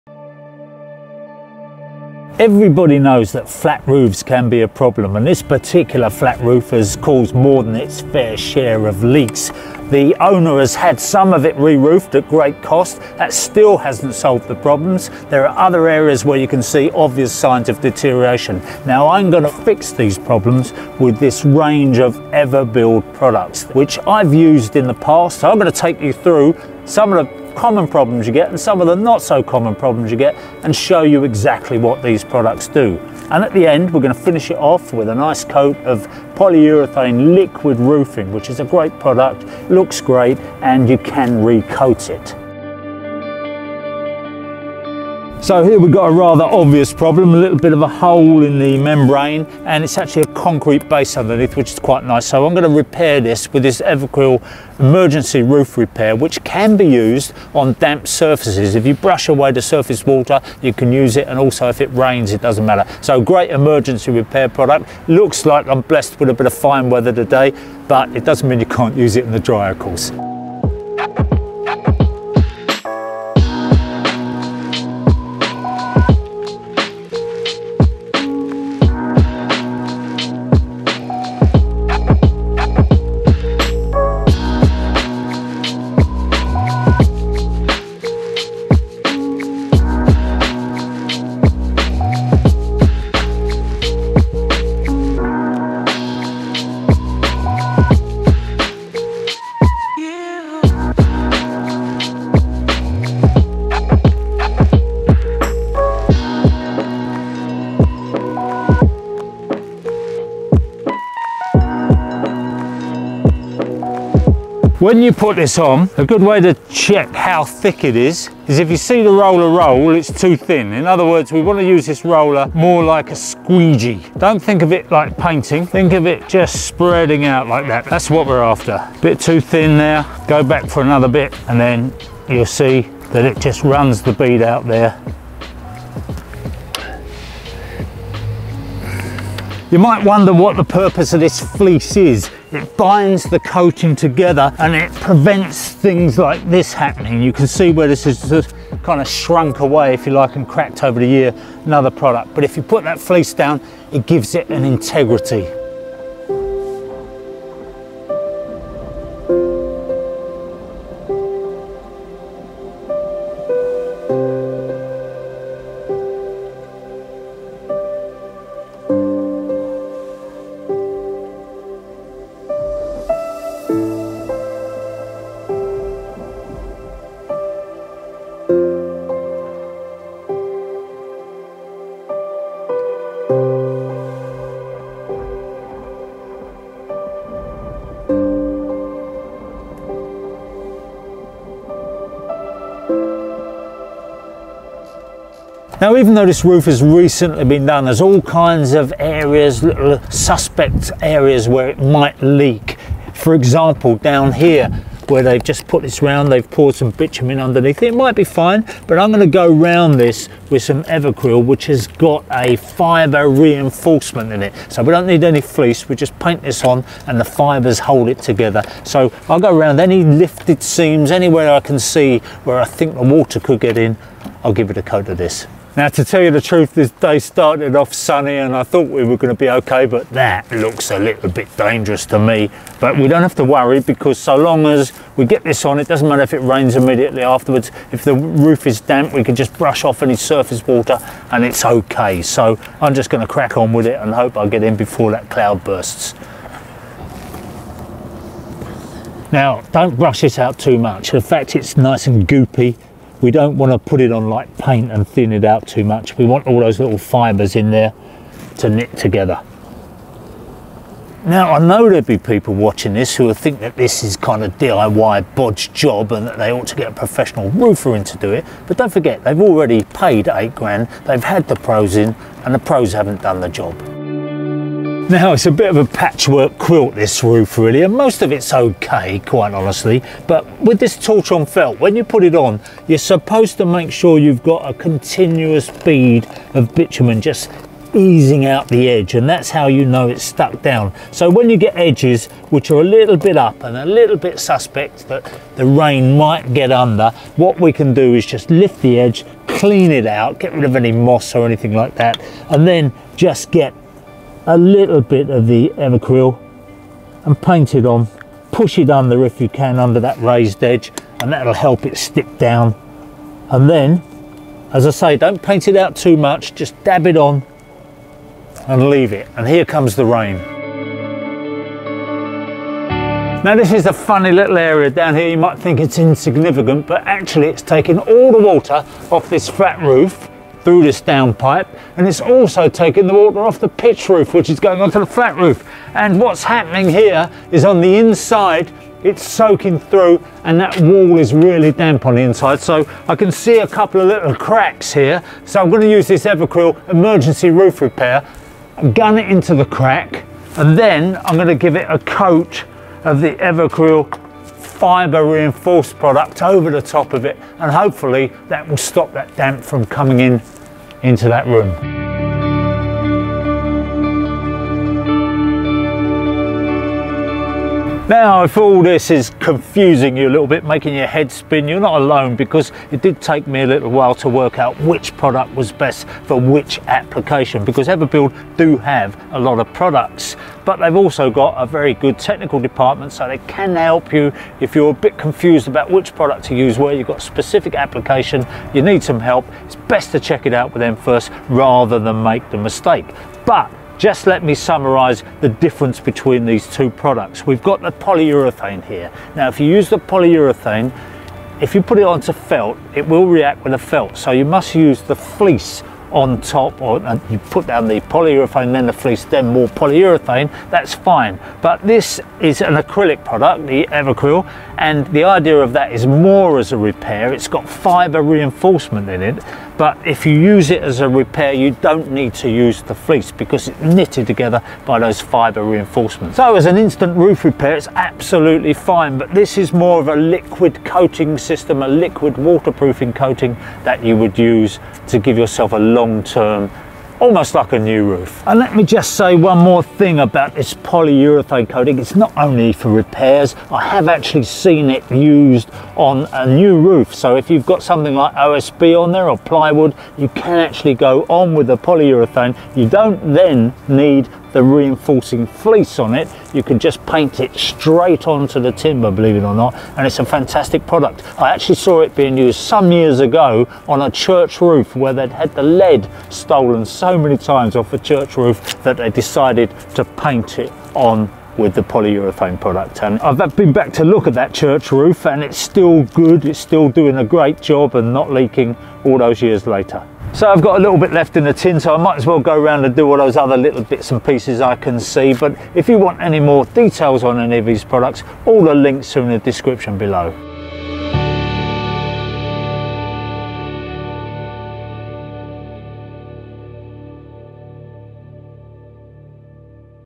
How to Repair a Flat Roof with Liquid Roof
Roger repairs a troublesome flat roof that's had several failed fix attempts.

Liquid Roof all-weather system for waterproofing any flat or pitched roof

JetVac Cleaning ~ Exterior cleaning specialist based in Canvey Island, Essex